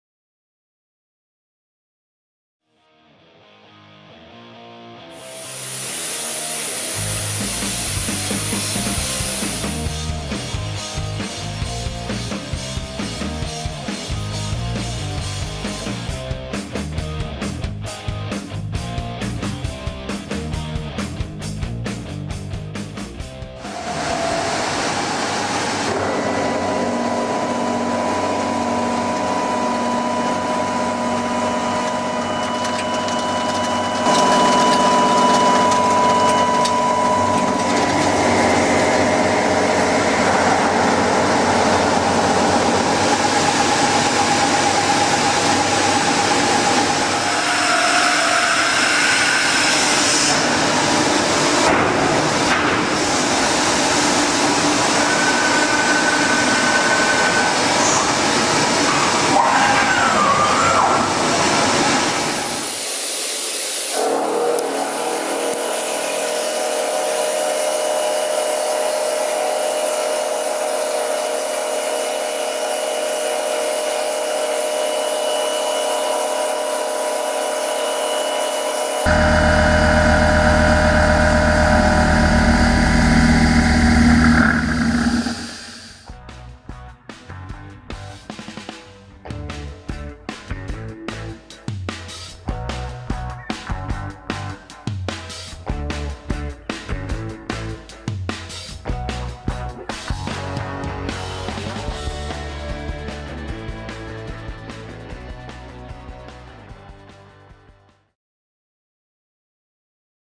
High volume cutting on Starrag STC 800
High volume cutting test on Starrag STC 800 simultaneous 5 axis machining center.
Efficient high performance machining requires a rugged machine build-up without weakness. All technical parameters such as axis strokes, forces, torques and accelerations are set according to application requirements.The design of the STC series has no bottleneck reducing the maximum possible material removal
rates (like motor spindles with slender shafts, long quills). With its design, unmatched productivity is possible.